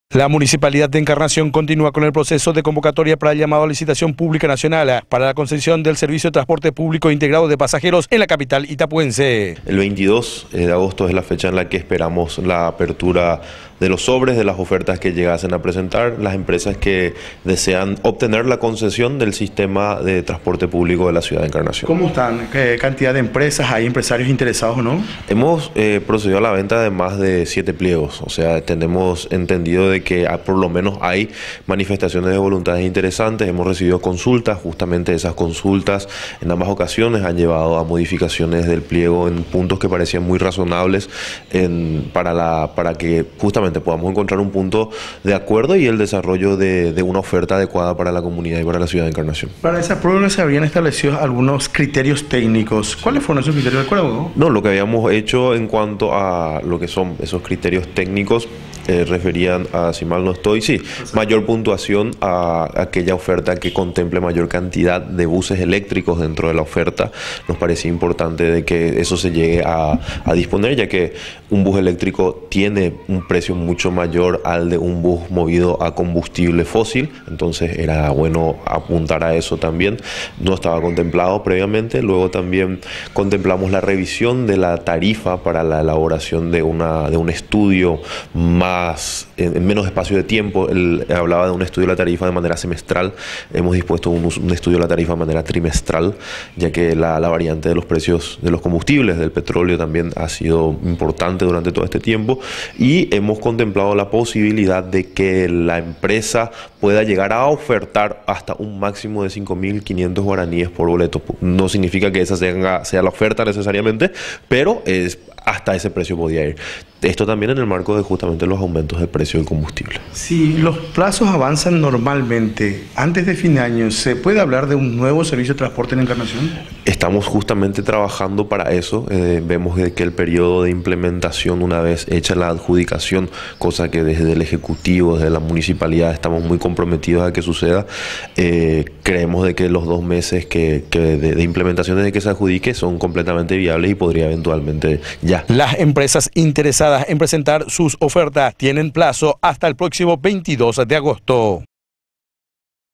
Transporte público: Hasta el 22 de agosto hay tiempo para presentarse a la licitación
El pliego de bases y condiciones detalla todos los datos concernientes a los requerimientos que debe cumplir la empresa interesada en prestar el servicio en nuestra ciudad.

Desde la Municipalidad, se remitieron invitaciones personalizadas a empresas locales y nacionales, como así también, la convocatoria fue difundida en tres diarios de circulación nacional, una revista especializada, y los medios de comunicación local y redes sociales a fin de garantizar la mayor cantidad de empresas informadas e interesadas de participar del llamado.

Los interesados tienen tiempo de presentación hasta el 22 de agosto próximo.